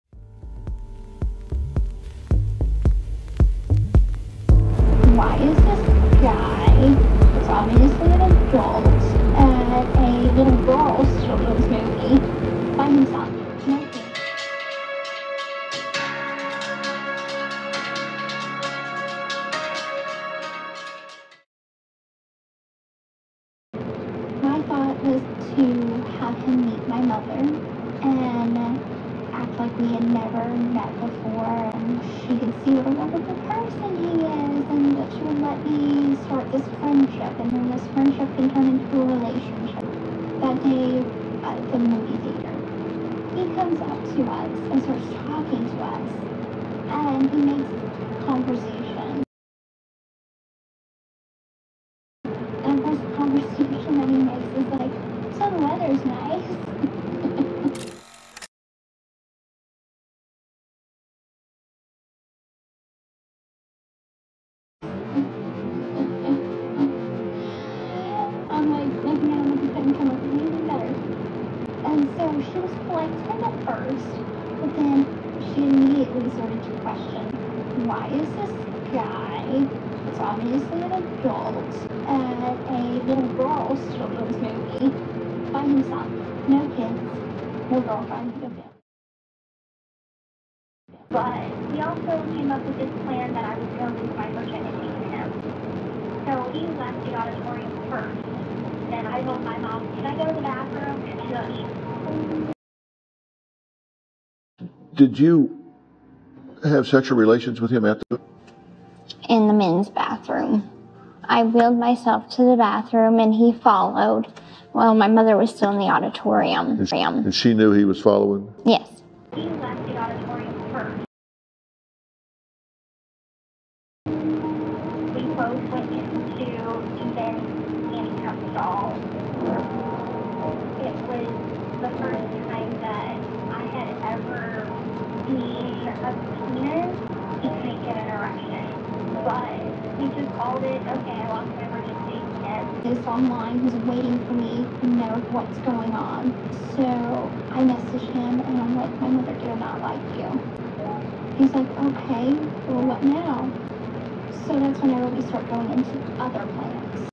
Gypsy Rose Tells a Different Story in New Documentary!
Gypsy Rose Blanchard has a new series on Lifetime where she light heartedly recalls her relationship with Nick Godejohn who is serving a life sentence for the murder of Gypsy’s mom, Dee Dee Blanchard.